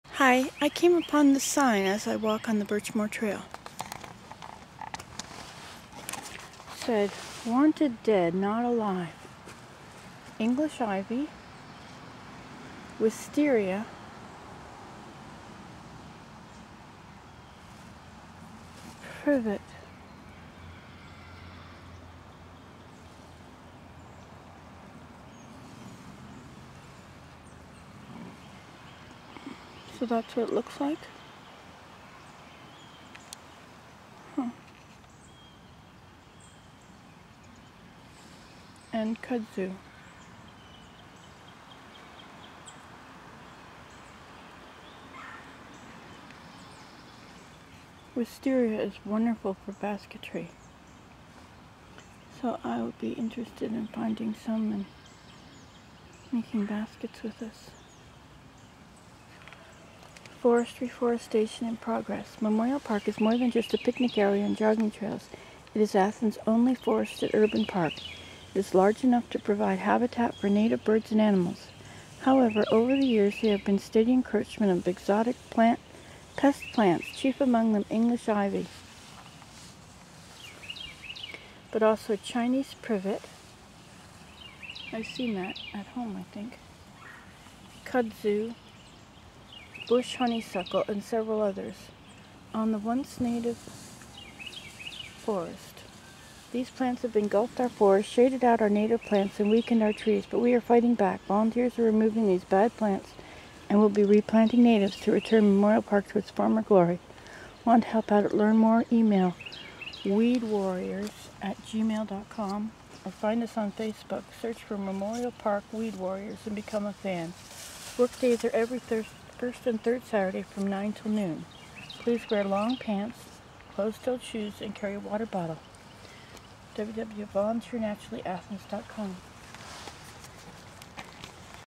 Nancy Today: Invasive species in Athens (wisteria 4) (Birchmore 14) ASMR
So nice to see the list with pictures so I can know what plants are which.  Walking in Memorial Park on the Birchmore trail.